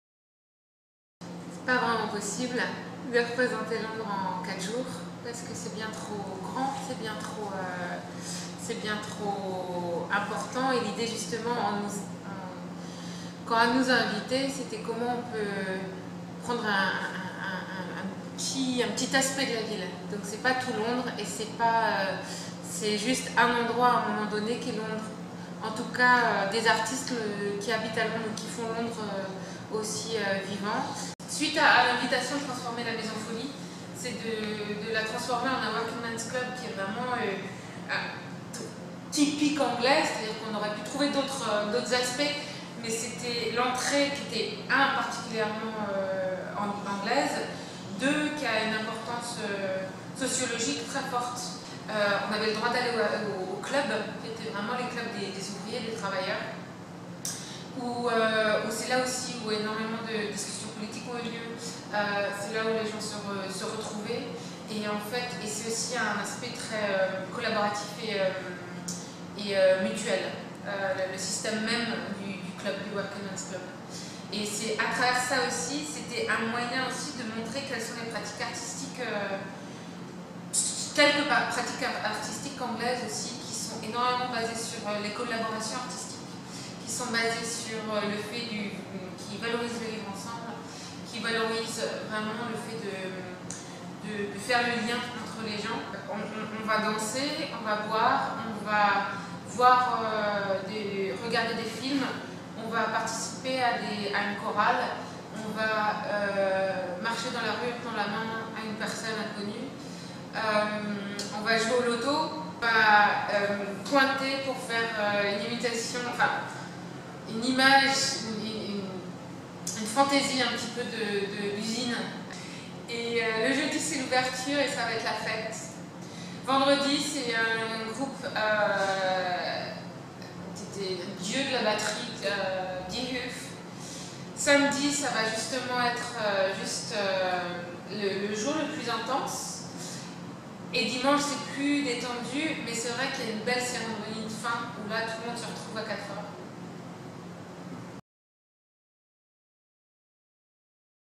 Ailleurs en Folie Londres - Interview avec Marine Thévenet
Marine Thévenet, commissaire de l'Ailleurs en Folie Londres, nous livre quelques informations sur l’événement. Du 19 au 22 février prochain, venez découvrir la capitale anglaise à la Maison Folie. Quatre jours qui promettent le voyage et la fête dans un esprit de communauté. Si vous souhaitez rencontrer Mavis Davies et ses Tabards, rendez-vous au Working Mons Club.